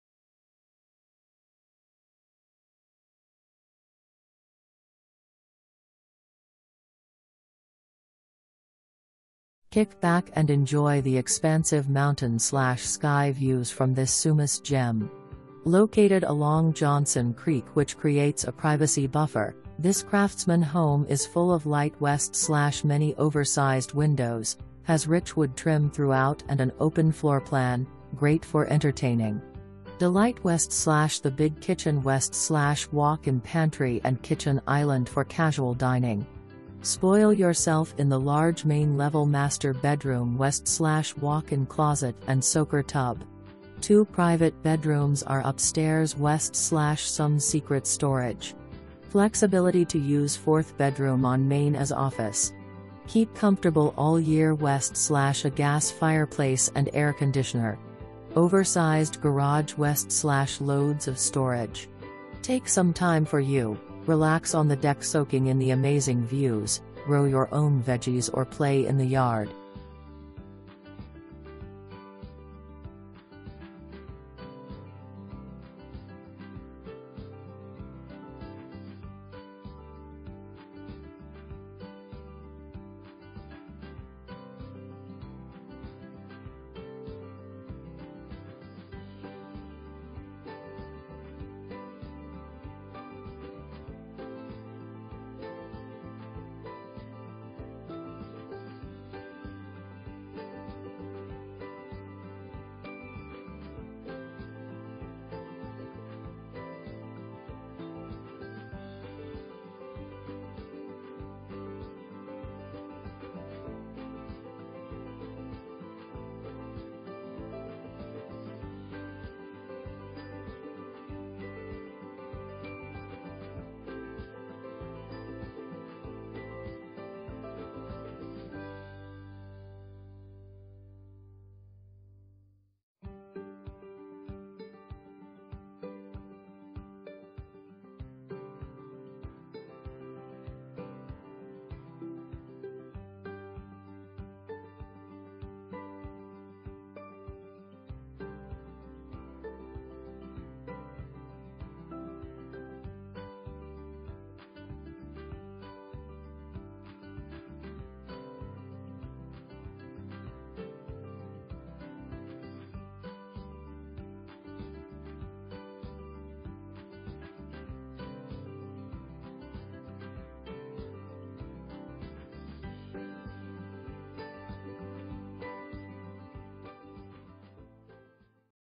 525 E 3rd St
525 E 3rd St
Sumas, WA 98295

Kick back & enjoy the expansive mountain/sky views from this Sumas gem! Located along Johnson Creek which creates a privacy buffer, this craftsman home is full of light w/many oversized windows, has rich wood trim throughout & an open floorplan - great for entertaining. Delight w/the big kitchen w/walk-in pantry & kitchen island for casual dining. Spoil yourself in the large main level master bedroom w/walk-in closet & soaker tub. 2 private bedrooms are upstairs w/some secret storage. Flexibility to use 4th bedroom on main as office. Keep comfortable all year w/ a gas fireplace & AC. Oversized garage w/loads of storage. Take some time for you: relax on the deck soaking in the amazing views, grow your own vegies or play in the yard.

2 full bath, 1 half bath

Leslie Hobkirk
Windermere Real Estate/Whatcom, Inc.